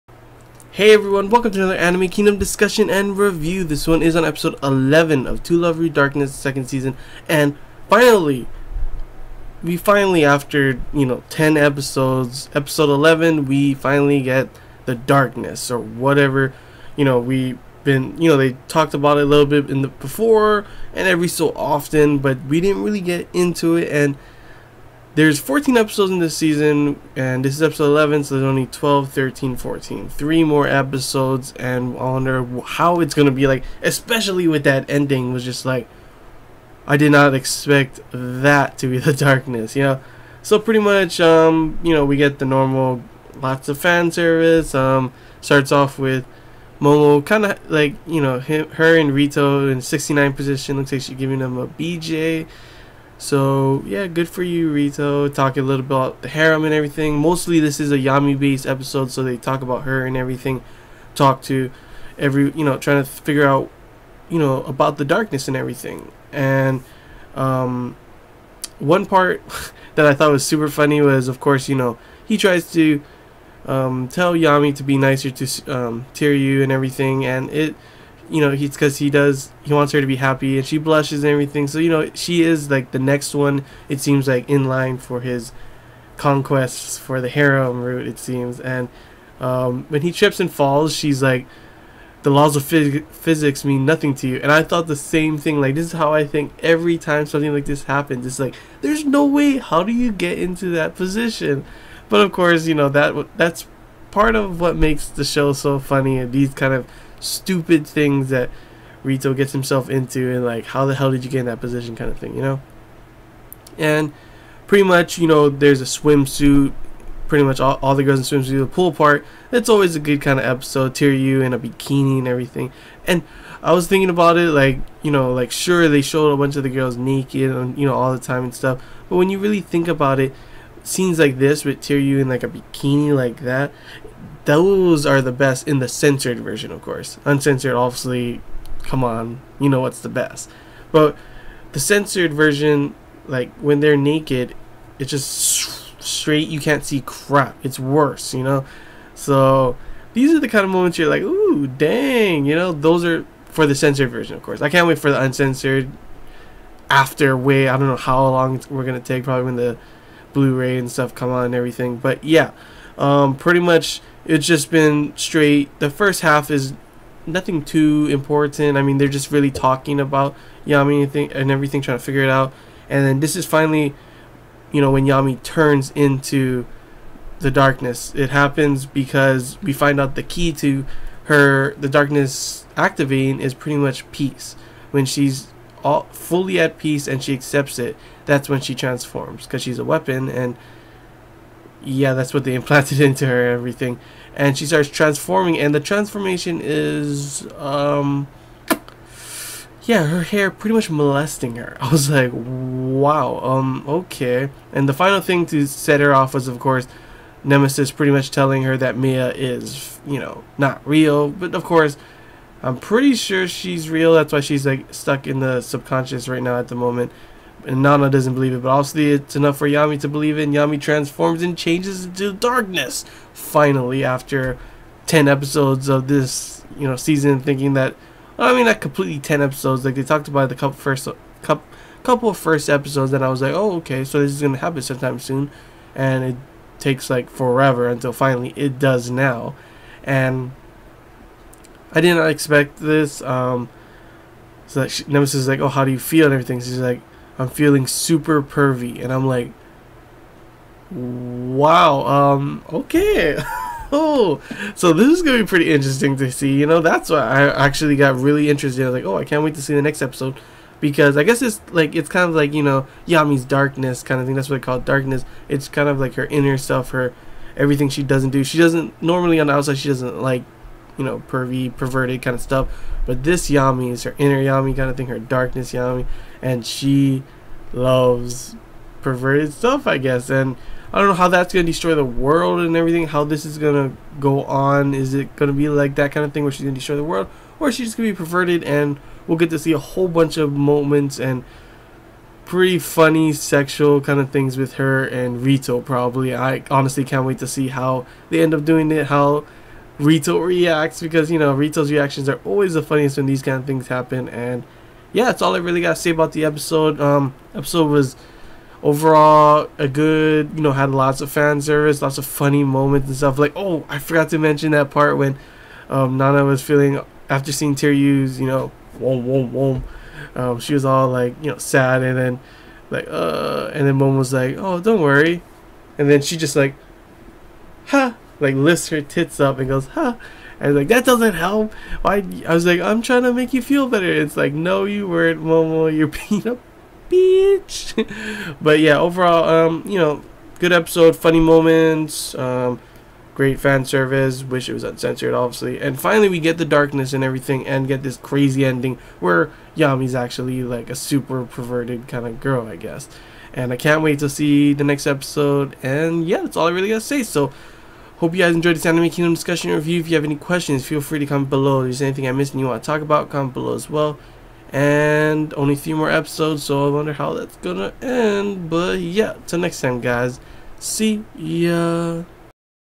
To LOVE-Ru Darkness 2nd Episode 11 Discussion & Review
Hey everyone! In this video I discuss what I saw and what I liked in episode 11 of To LOVE-Ru Darkness 2nd. If i missed anything then please comment below since this is a discussion we can talk about it :D Hope you guys enjoy this video and if you do give the video a like and don't forget to subscribe for more content! :D